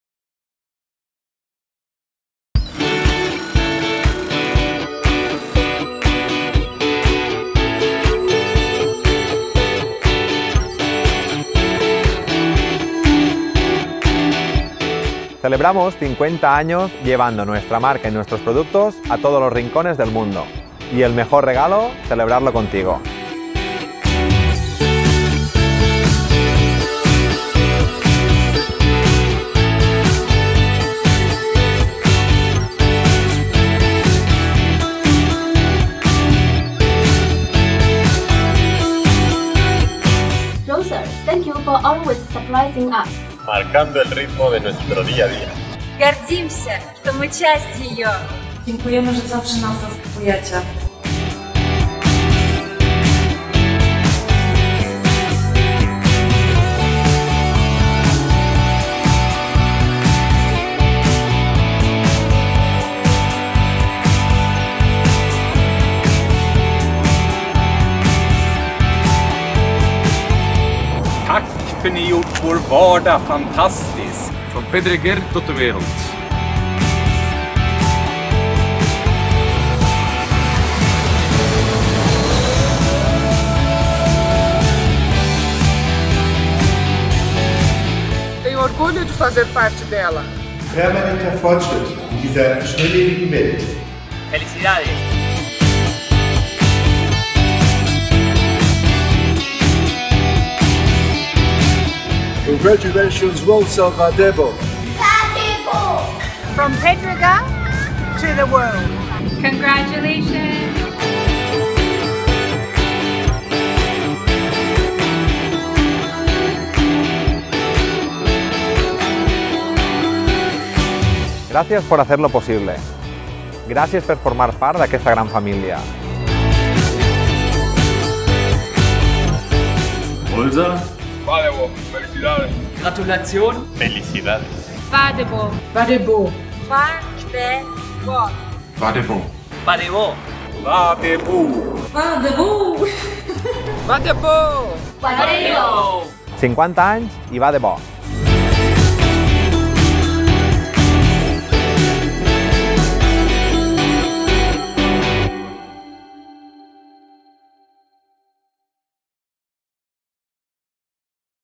50th Anniversary Rolser. Va de Bo !
Another member of the family, 50 years later!!

For the past 50 years, Rolser has been delivering innovated shopping trolleys to thousands of homes over 5 continents.

Significant advances are often obtained thanks to small details. They often become so familiar that we forget what they contribute, as well as the time they have been by our side.

Well... all of 50 years. The shopping trolley, the ironing centres and the ladders, for home and professional use, aside from their essential role in our day-to-day lives, all have in common the company which manufactures them and helps them reach our homes: ROLSER.
In 2016, this Pedreguer (Alicante) company is celebrating its 50th anniversary; half a century working and building the company from this small town of about 7,000 inhabitants to gain presence in the main window displays all over the world.
In the early days, ROLSER was dedicated to the artisan manufacturing of palm baskets. The activity evolved towards an increasingly demanded product as is the shopping trolley, forcing the members of the family to take on the challenge of building new facilities which have gradually increased in size until becoming the manufacturing facilities we see today, with the company headquarters which can be found in Pedreguer itself and currently spreading over 21,000 m2.
The shopping trolley, the emblematic product which is known all over the world, is an example of ROLSER's entrepreneurial spirit, always seeking innovation, contributing improvements and evolving to make day-to-day tasks easier, with high quality and design as the differential values enabling ROLSER to take the lead in this demanding market sector.
Despite reaching a milestone of 50 years creating commercial successes, at ROLSER this is looked upon with a natural approach and a touch of non-conformism, yet with satisfaction for a job well done. A critical and tireless spirit which is reflected in the slogan accompanying the 50th anniversary logo: “Va de bo”; a colloquial expression from the Alicante region used for describing the desire to gain momentum, to always take things seriously, for demanding a wake-up call for oneself to continue with the same impetus and for achieving new objectives.
50 years on, the company founded by Vicente Server Ferrer and then taken on by his children Joan, Salvador and Paquibeles, is now an international benchmark for growth and expansion of products and politics which look towards the future. But maintaining the roots, the desire to go further: “Va de Bo!”